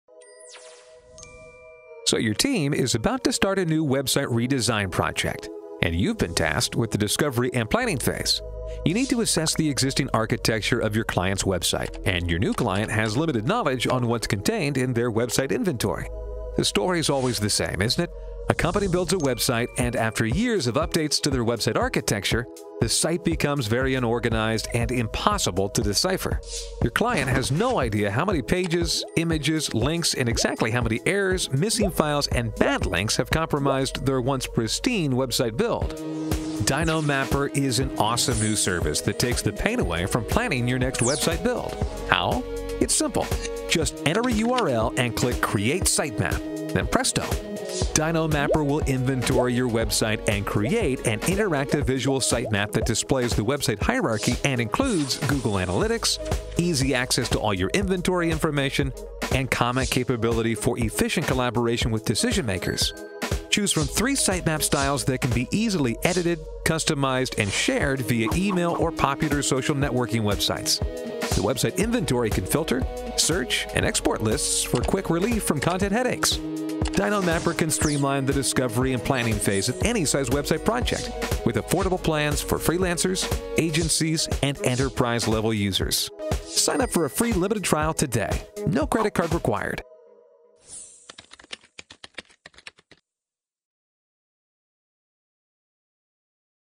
DYNO Mapper® - Sitemap Generator
The DYNO Mapper® sitemap generator is used by web designers, web developers, user experience architects, and project managers. DYNO Mapper® creates an interactive sitemap that displays your website's hierarchy with easy access to website inventory, Google Analytics integration, and comment capability for efficient collaboration. Signup for a free 14 day trial - start creating sitemaps today.